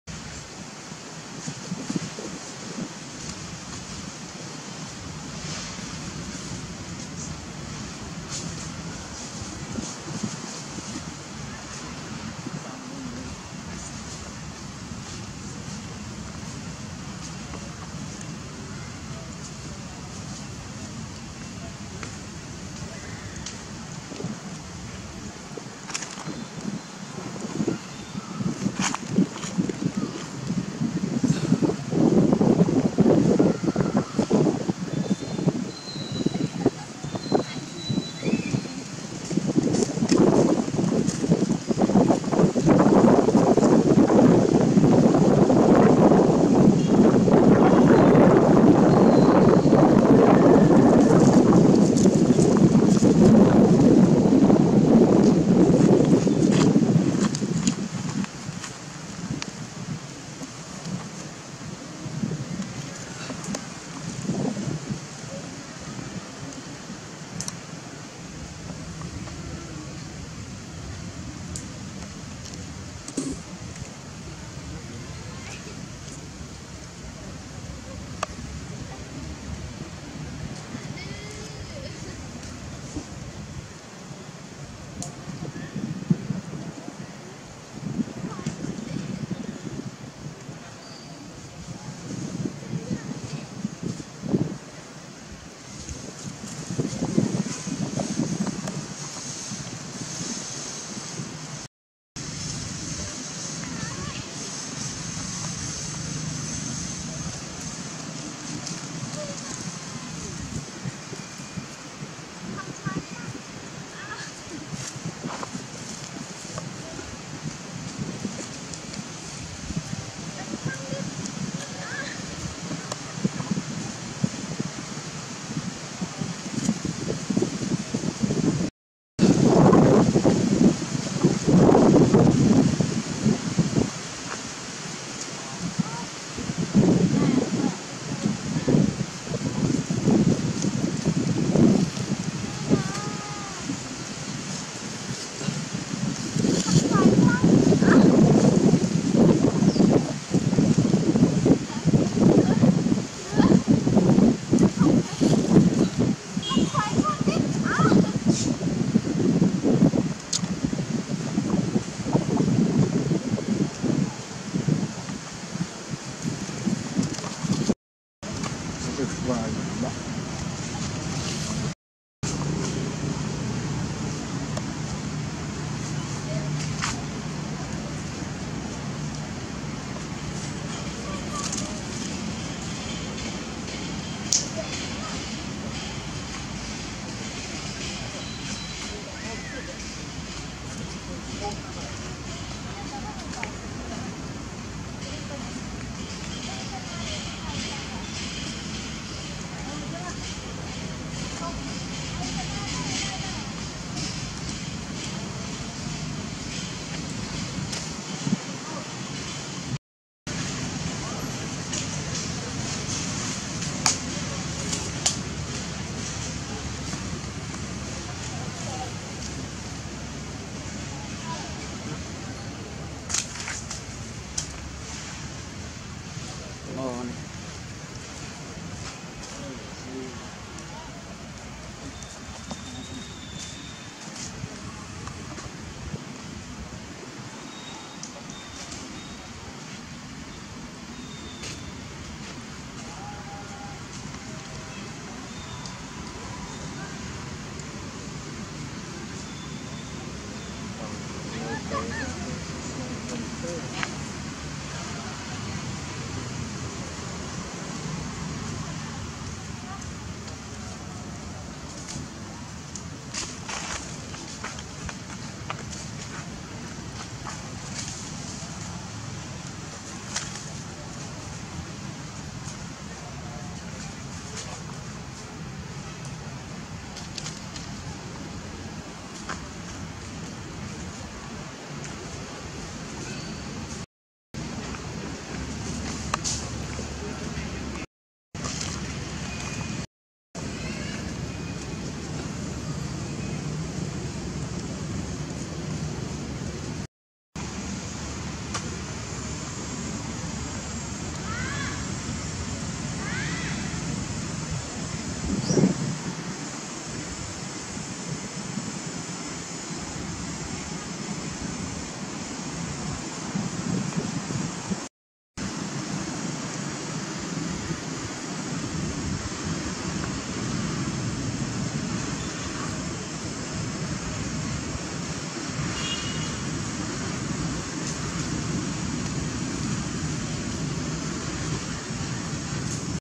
Poor tiny monkey Jessie got a mango | Orphan monkey Jessie hungry and eating delicious
If you love watching monkey videos  we will upload more.